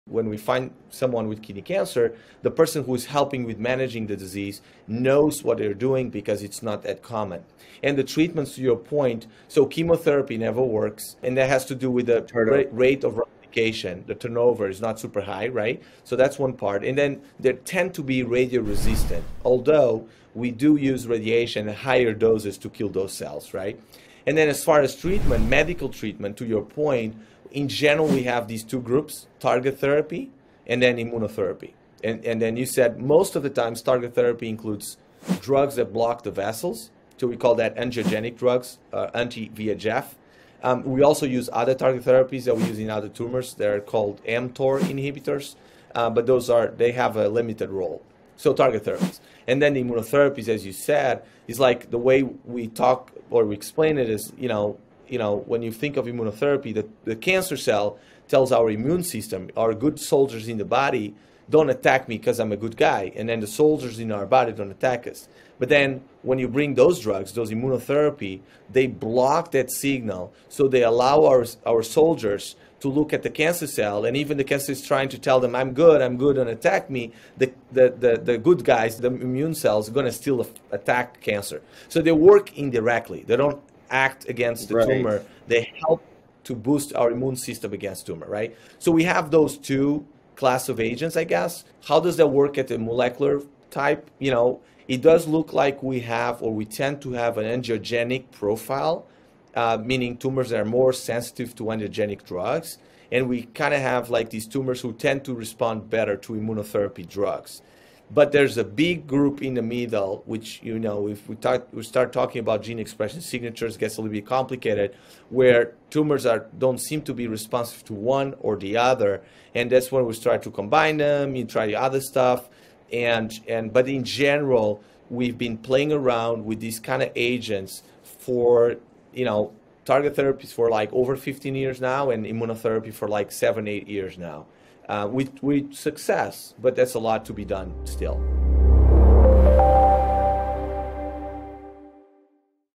Treatment options for kidney cancer
Chemotherapy? Radiation? Targeted therapy? Immunotherapy? Which treatments are being used to treat kidney cancer? Dr. Pedro Barata, Associate Professor of Medicine at Case Western University and a leading expert in genitourinary tumors, explains the options.